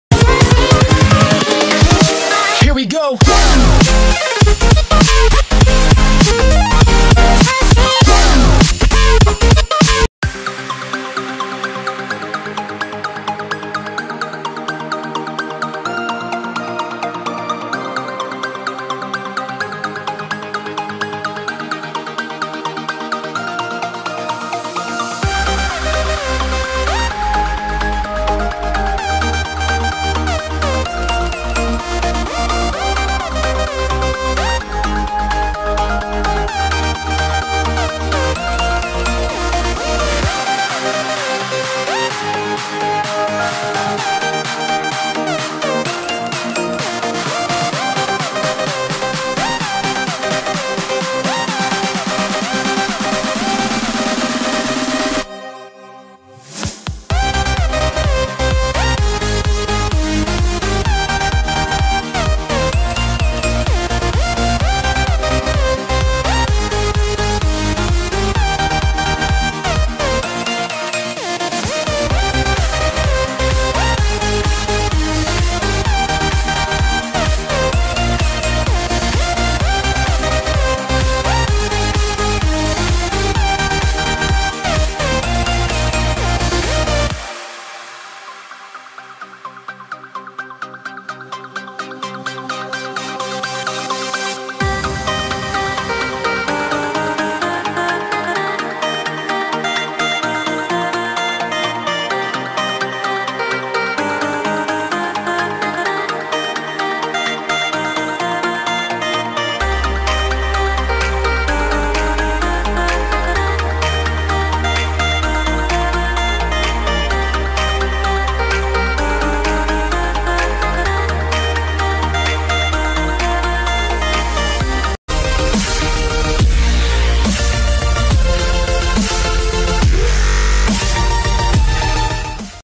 Vian­ney arrête la musique ? « J'ai juste besoin de me repo­ser
Inter­viewé par nos confrères de Télé Star, Vian­ney est revenu sur le succès de son deuxième album et son envie d'arrê­ter momen­ta­né­ment sa carrière pour se repo­ser.L'homme a besoin de repos.Fort de son succès, Vian­ney a fait son trou dans la chan­son française.Avec un premier album certi­fié ...